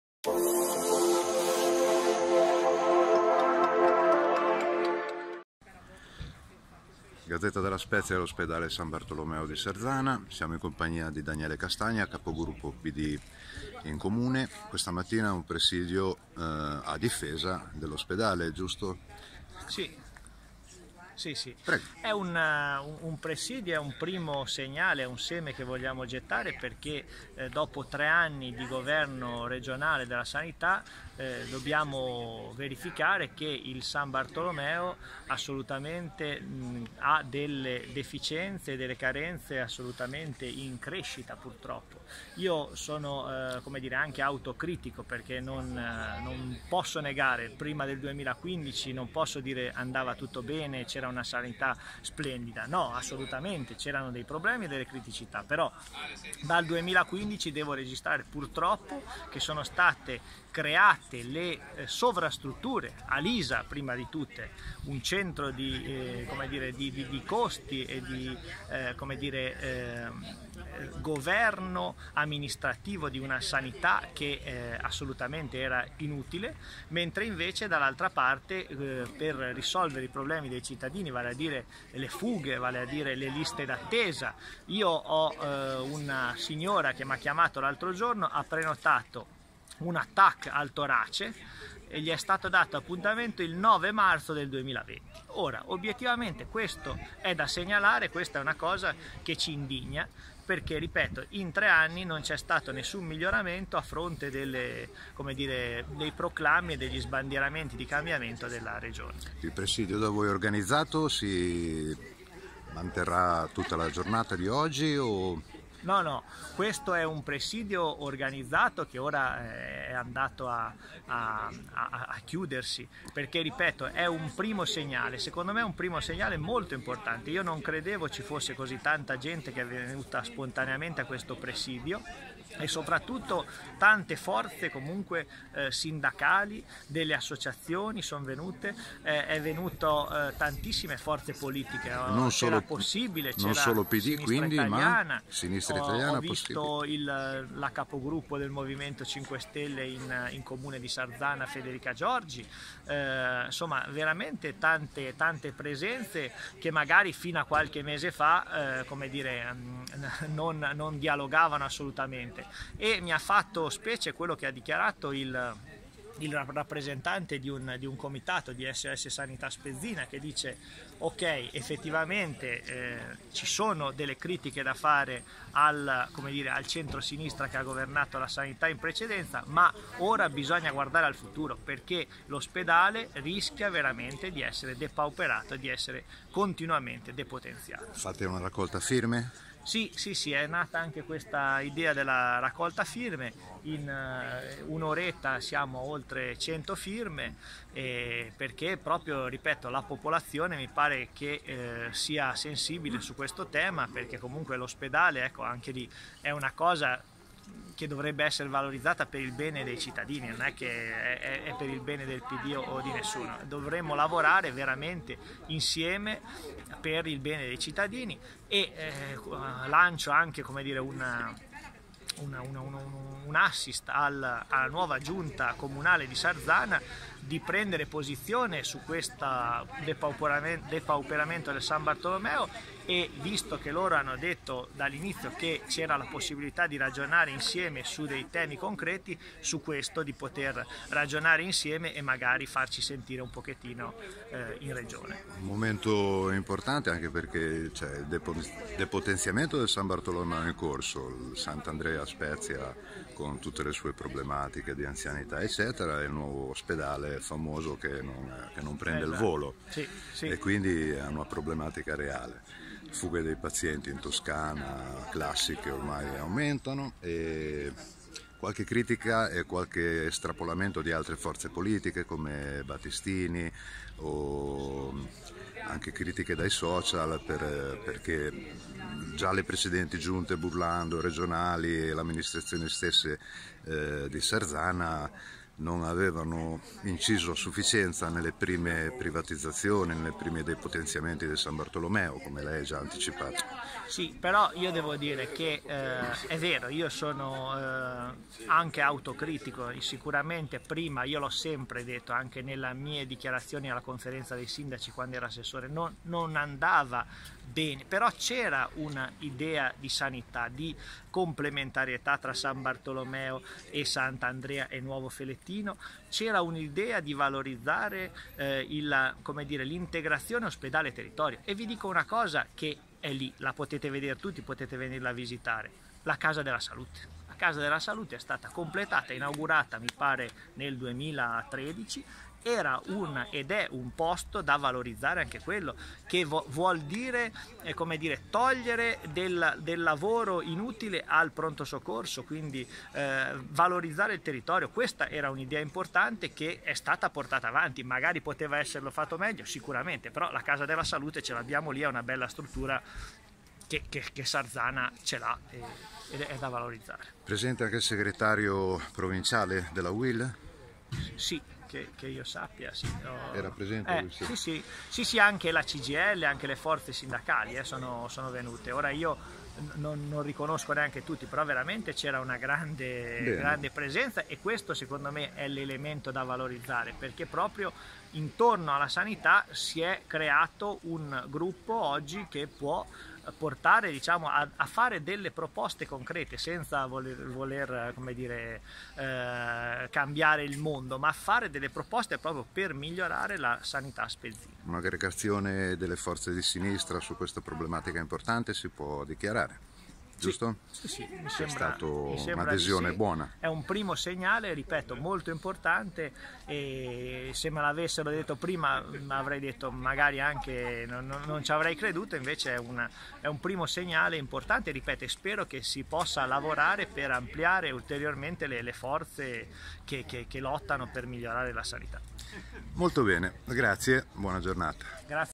Presidio di fronte all'ospedale San Bartolomeo
Questa mattina, 28 agosto, presidio con raccolta firme del Partito Democratico all’ingresso del San Bartolomeo, prima tra le iniziative che seguiranno in tutti i Comuni della Provincia.
L’iniziativa è partita dall’Unione comunale Pd Arcola il cui segretario, Carlo Canese, era presente al presidio. Insieme a lui il regionale Juri Michelucci, Giovanni Lunardon segretario regionale PD, l’ex Sindaco Alessio Cavarra, il Consigliere Daniele Castagna, l’ex Consigliere Paolo Zanetti ed il segretario generale della Uil Liguria, Marco Ghini. Hanno aderito forze politiche, sindacali e associazioni; Sinistra Italiana – Possibile - Cgil – Uil – Arci- Uisp – Camera del lavoro di Sarzana – S.O.S Sanità Spezzina. Sono passati a portare la loro testimonianza al presidio anche il Consigliere comunale, Federica Giorgi (M5S) e Valter Chiappini (Associazione Sarzana in movimento)

Ricevi tutte le sere le notizie del giorno gratuitamente sulla tua email!

Contatti:

Redazione: Via delle Pianazze, 70 – 19136 La Spezia